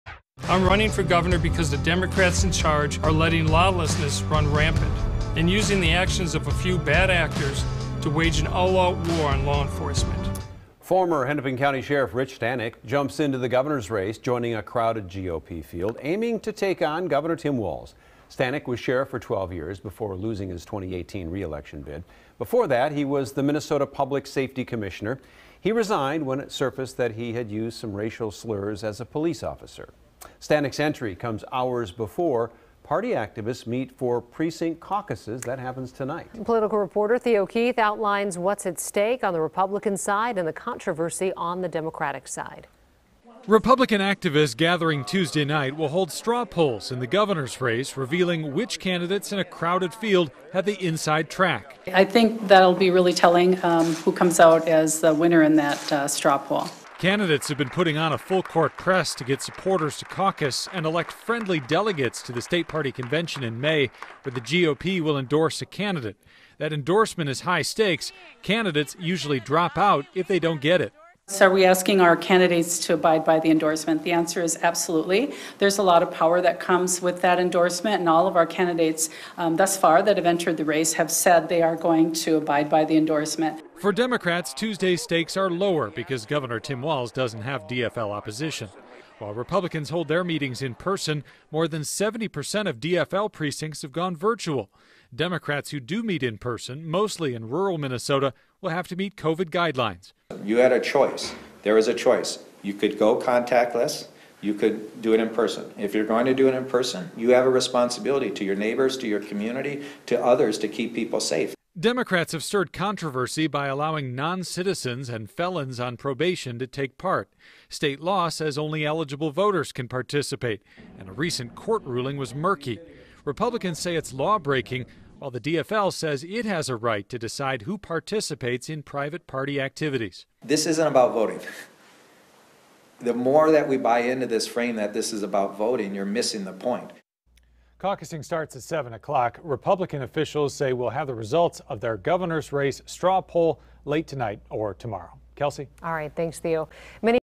Rich Stanek, former Henn. Co. Sheriff, announces run for governor | FOX 9 KMSP
Former Hennepin County Sheriff Rich Stanek jumped into the race for Minnesota governor on Tuesday, joining a crowded Republican field vying to take on Gov. Tim Walz this fall.